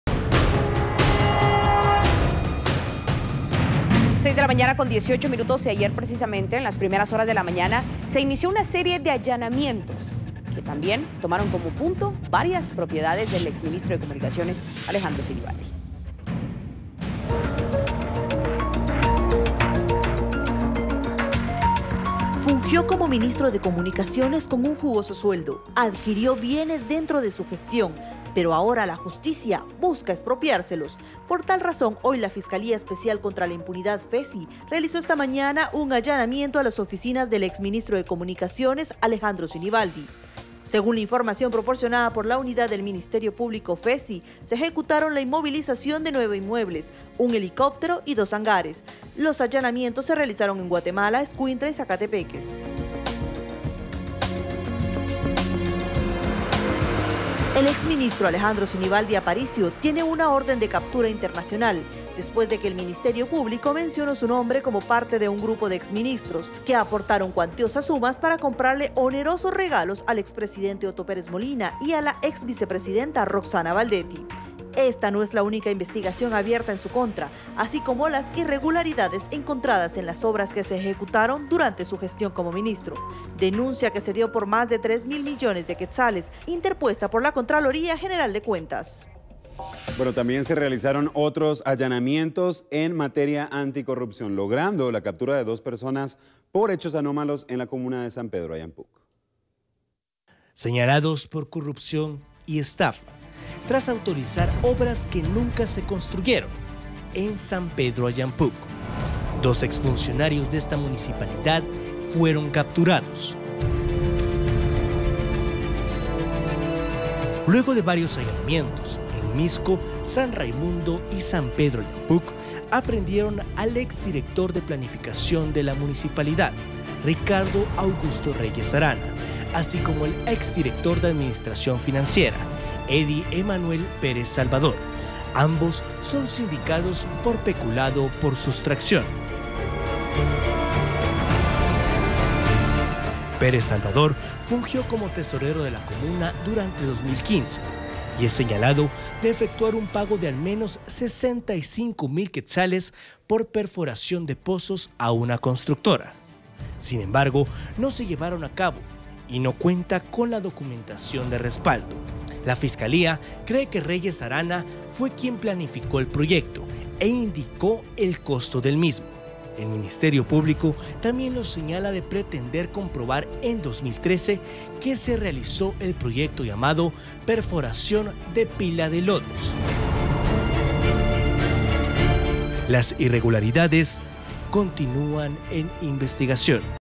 Allanamientos contra la corrupción
El día de ayer se realizaron varios operativos de la FECI en todo el territorio nacional, principalmente en la ciudad capital, en donde resaltan los allanamientos contra propiedades del ex Ministro de Comunicaciones, Alejandro Sinibaldi. Además resaltó la captura de dos ex funcionarios de la Municipalidad de San Pedro Ayampuc. El ex funcionario Alejandro Sinibaldi sigue prófugo de la justicia